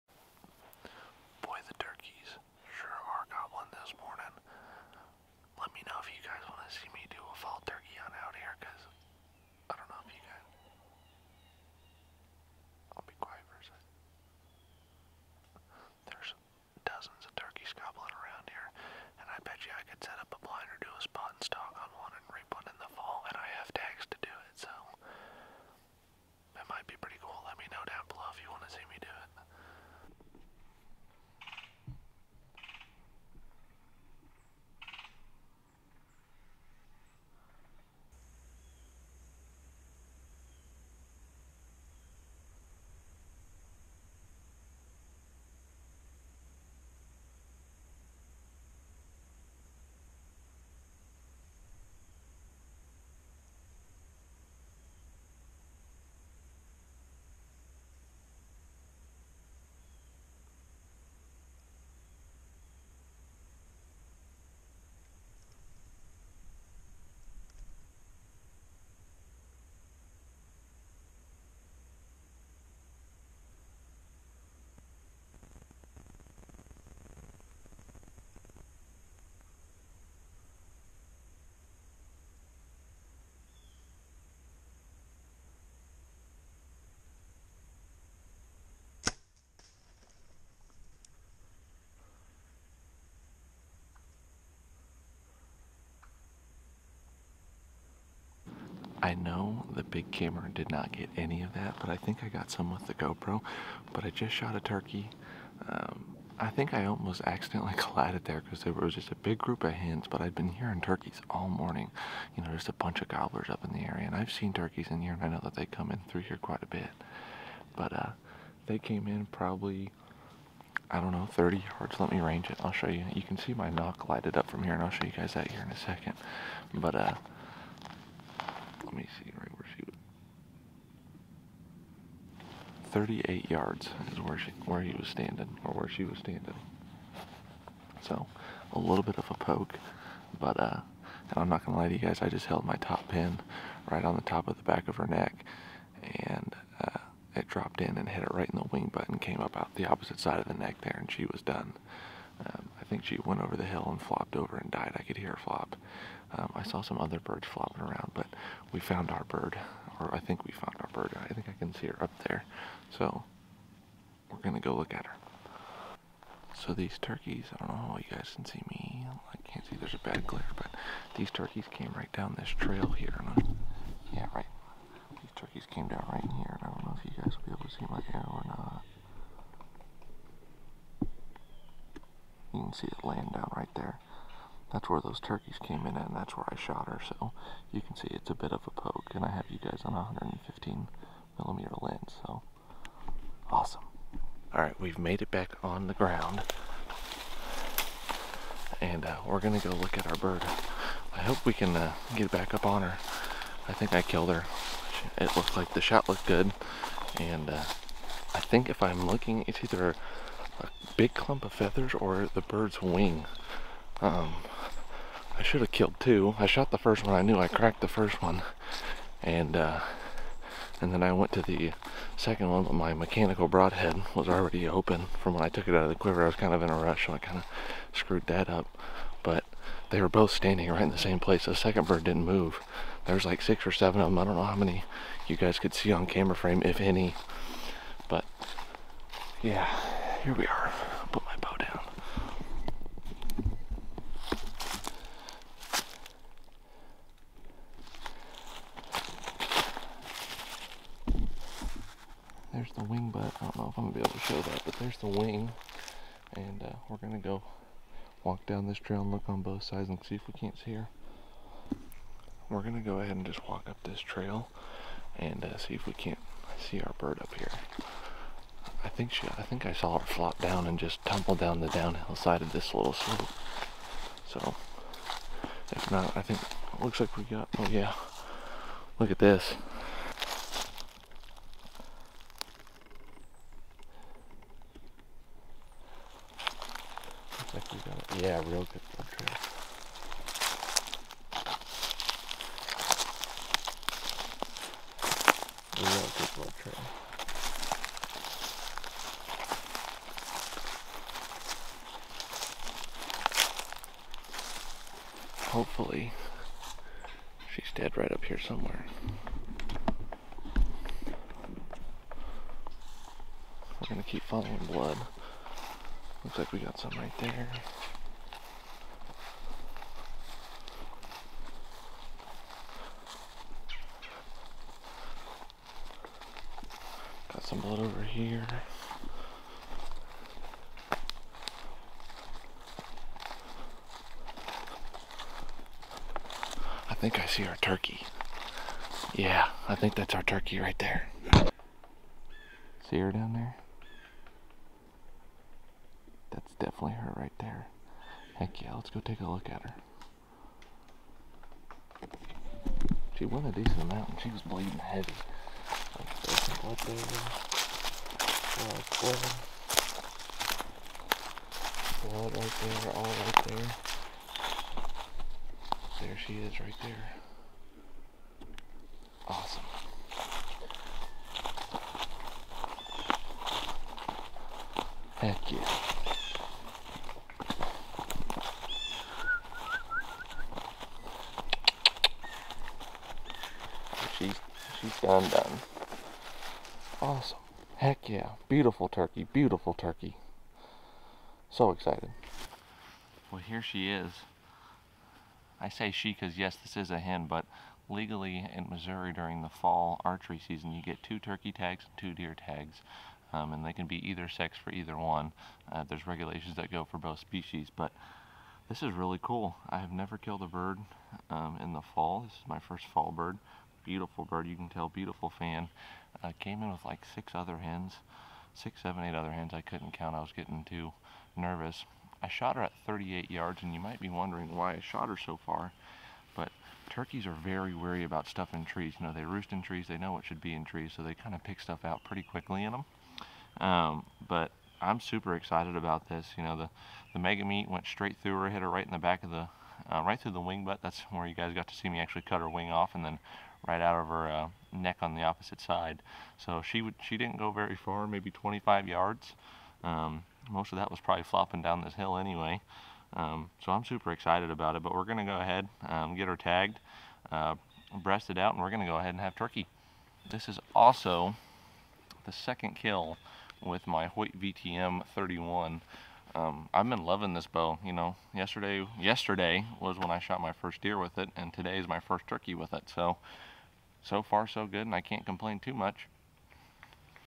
Bowhunting Public Land Turkeys In The Fall!
I Shot My First Turkey In The Fall! I have Bowhunted Public Land Turkeys Before But Have Killed One! I am super excited to show you guys My Hunt and show you the footage I managed to get! I have a new gopro that will be here for the next P.O.V shot that is newer and updated! Bowhunting Public Land Turkeys In The Fall is So much Fun!

Big Camera
Main Lens
Backup Lens
Action Camera
Batteries for Big Camera
Camera Protector